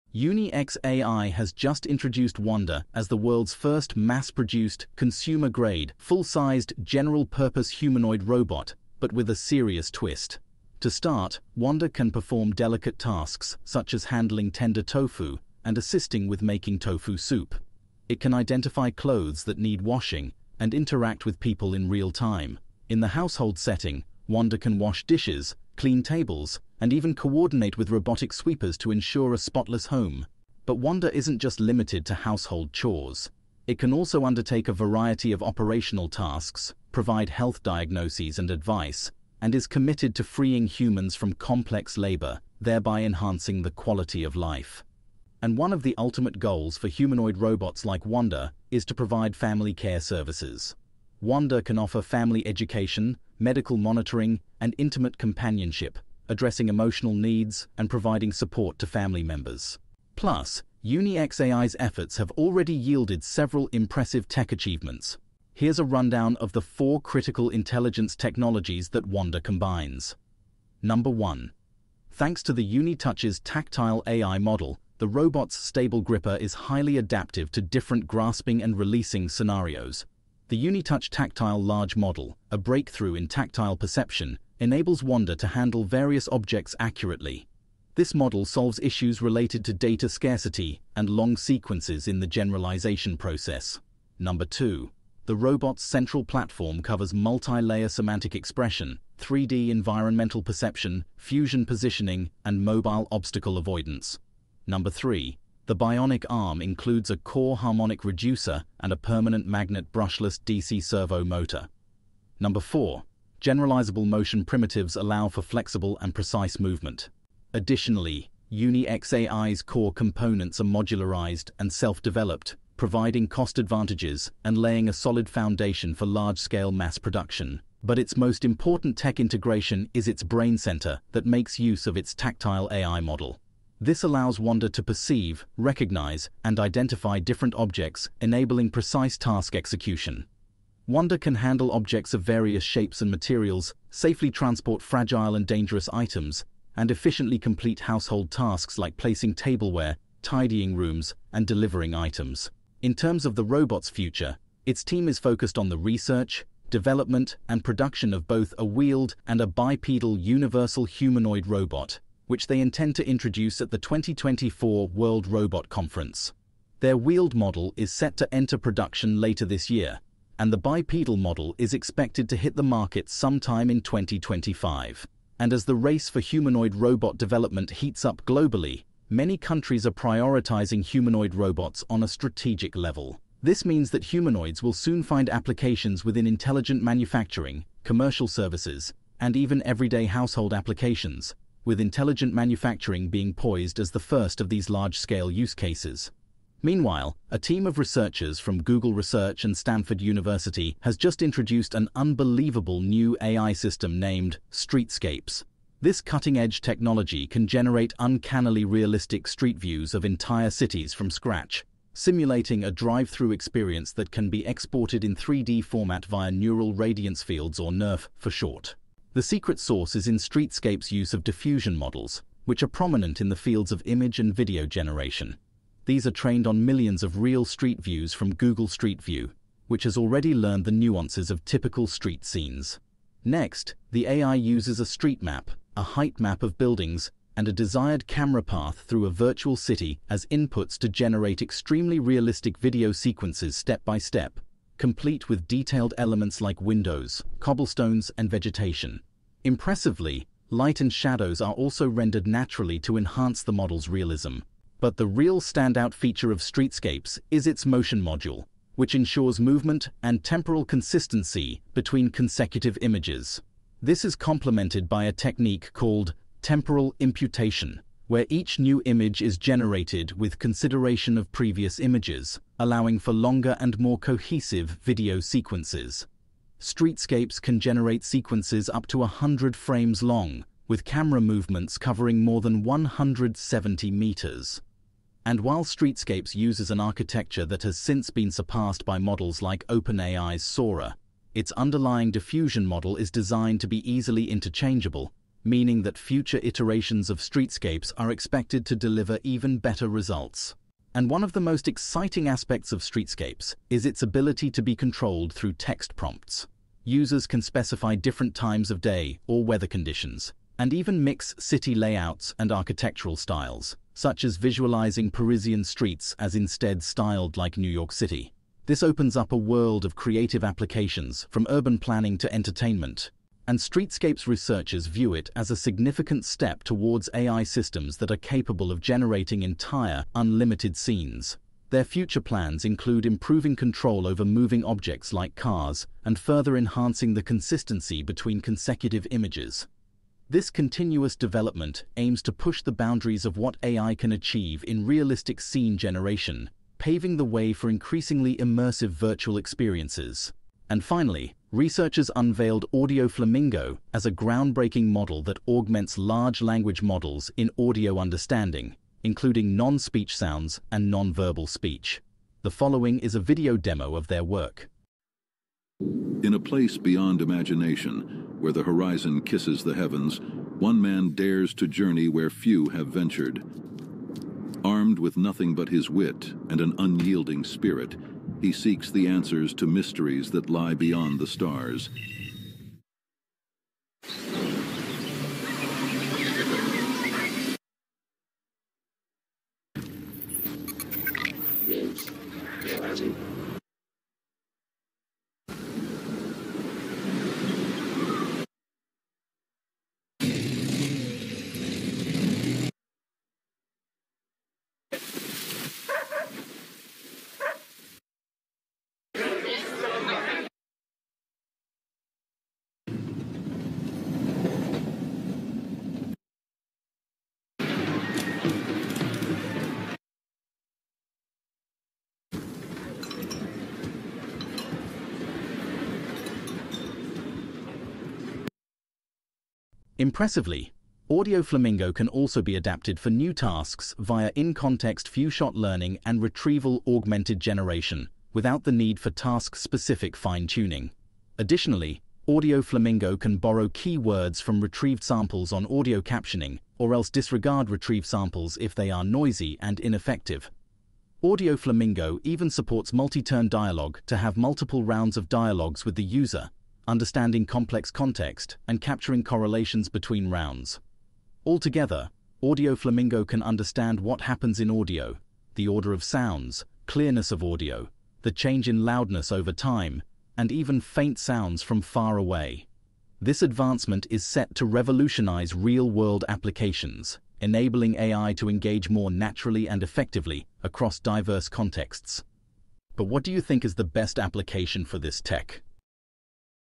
New UniX AI Robot Unveiled: Ultra "UniTouch" Intelligence Plus 32 DoF (GOOGLE + STANFORD AI)
Wanda the AI powered humanoid robot from China just demoed its new intelligence capabilities. Google and Standford release a new Streetscapes, and Nvidia enables Audio Flamingo.

AI news timestamps:
0:00 Wanda AI robot
3:17 Streetscapes
5:51 Audio Flamingo